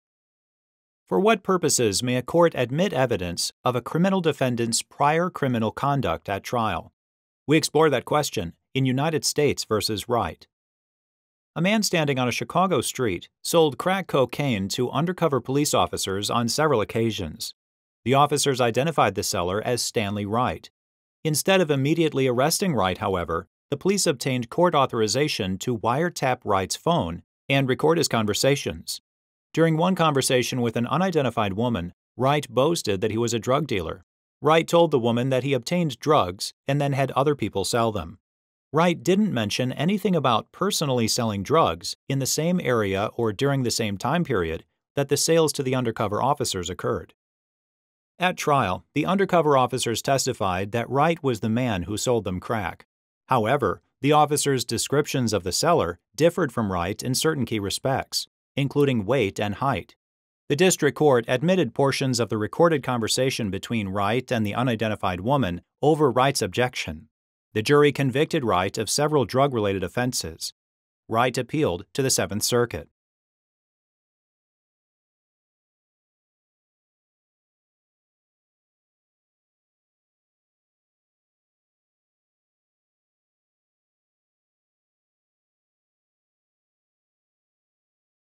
United States v. Wright Case Brief Summary | Law Case Explained
United States v. Wright | 901 F.2d 68 (1990)

For what purposes may a court admit evidence of a criminal defendant’s prior criminal conduct at trial? We explore that question in United States versus Wright.

A man standing on a Chicago street sold crack cocaine to undercover police officers on several occasions. The officers identified the seller as Stanley Wright. Instead of immediately arresting Wright, however, the police obtained court authorization to wiretap Wright’s phone and record his conversations. During one conversation with an unidentified woman, Wright boasted that he was a drug dealer. Wright told the woman that he obtained drugs and then had other people sell them. Wright didn’t mention anything about personally selling drugs in the same area or during the same time period that the sales to the undercover officers occurred.

At trial, the undercover officers testified that Wright was the man who sold them crack. However, the officers’ descriptions of the seller differed from Wright in certain key respects, including weight and height. The district court admitted portions of the recorded conversation between Wright and the unidentified woman over Wright’s objection. The jury convicted Wright of several drug related offenses. Wright appealed to the Seventh Circuit.

Want more details on this case? Get the rule of law, issues, holding and reasonings, and more case facts

Have Questions about this Case? Submit your questions and get answers from a real attorney